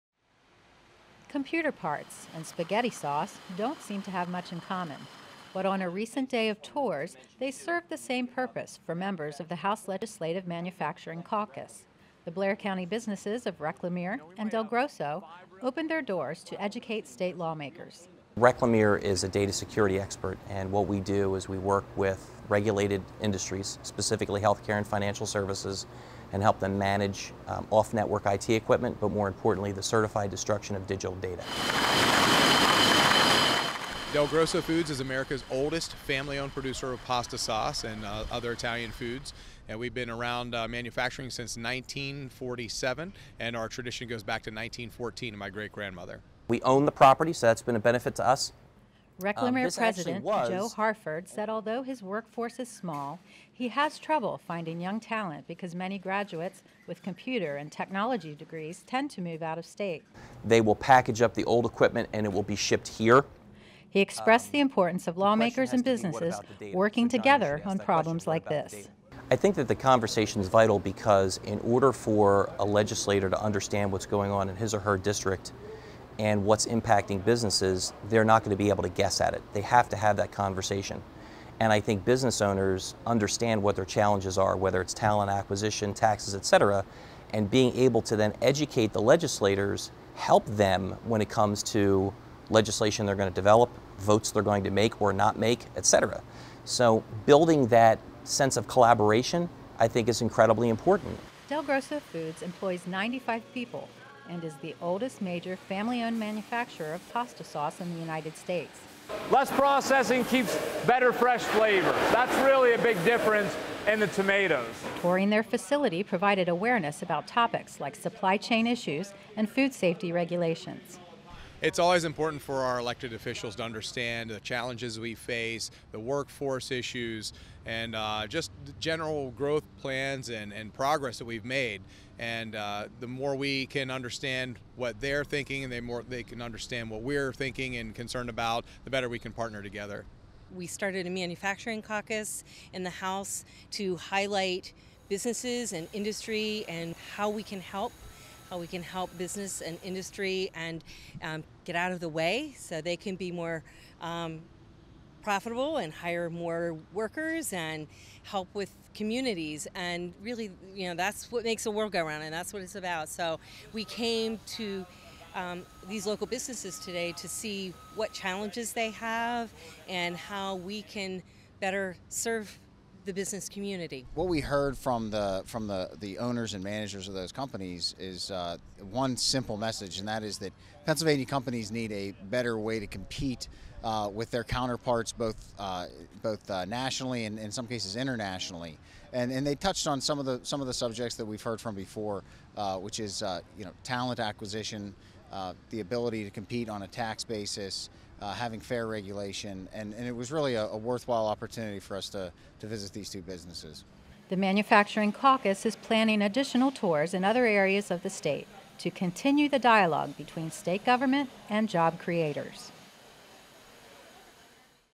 Lawmakers and Businesses Working Together
The House Manufacturing Caucus receives a tour of Reclamere and Delgrosso Foods.